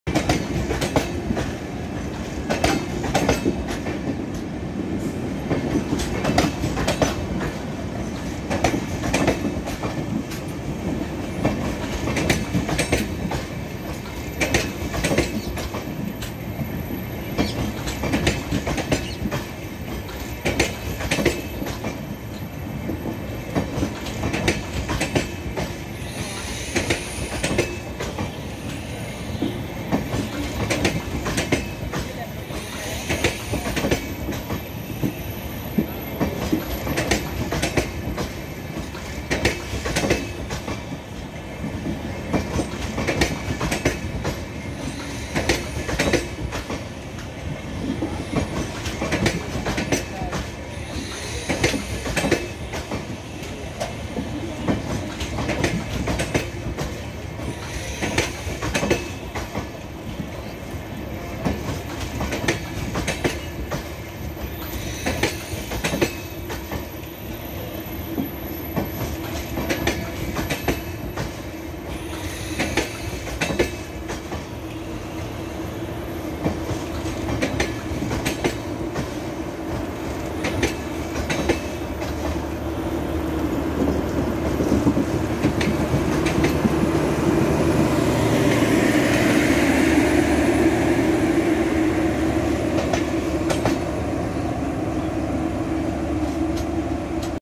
12074 Bhubaneswar Jan Shatabdi 1st LHB Run!!!!
It's time we take a few steps towards restoring normalcy after 4 consecutive national lockdowns.

So, from 1st June, 2020, Indian Railways started operating 100 pairs of regular trains with special numbers. 12074/3 Bhubaneswar Howrah Bhubaneswar Jan Shatabdi became the first train of East Coast Railway to start operations that too with LHB Rakes!

Seen here is: 37115 VSKP WAP7 departing Cuttack with the train. This is the original video and owner's channel.